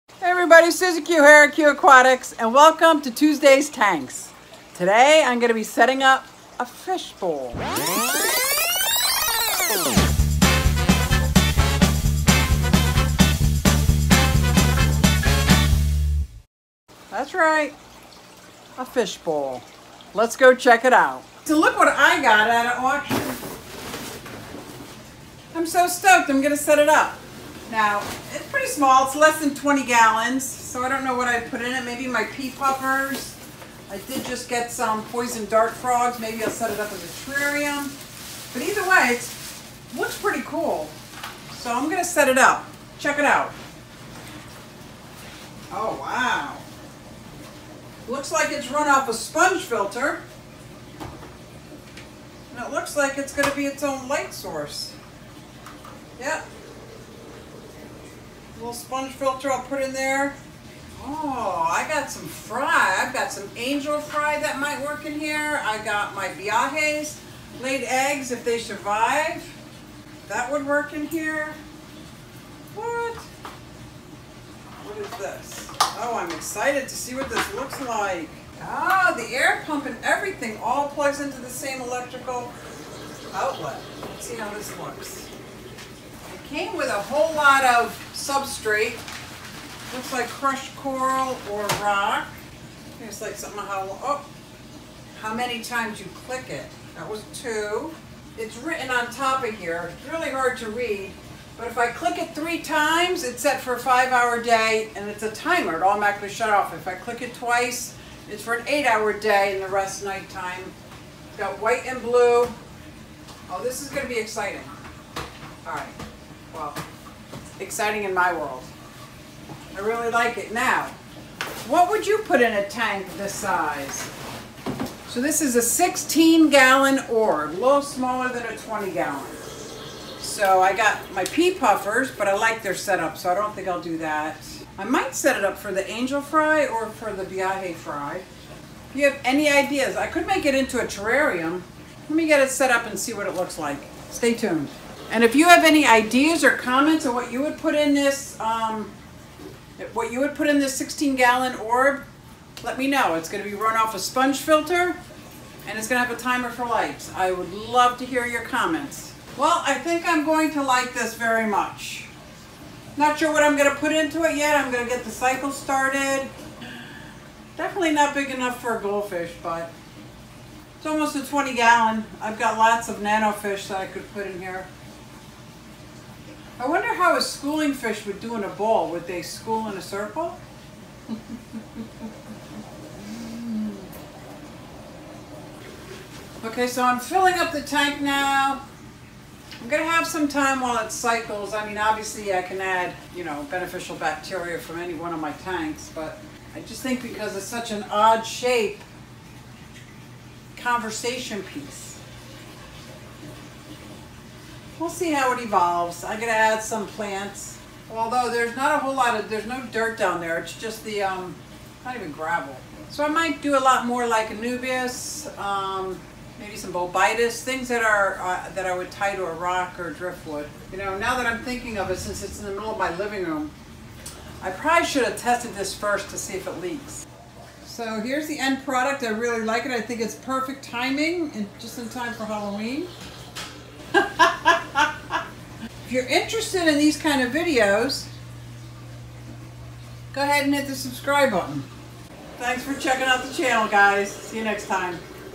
Biorb Aquarium Set Up - Fishless Cycle
Biorb Aquarium Set Up - Fishless Cycle. I am going to set up the biorb that I got at my fish club monthly auction. I need some ideas for what fish to stock. I was going to use some media from another tank to speed up the cycle but decided to not do that this time. At least not yet :)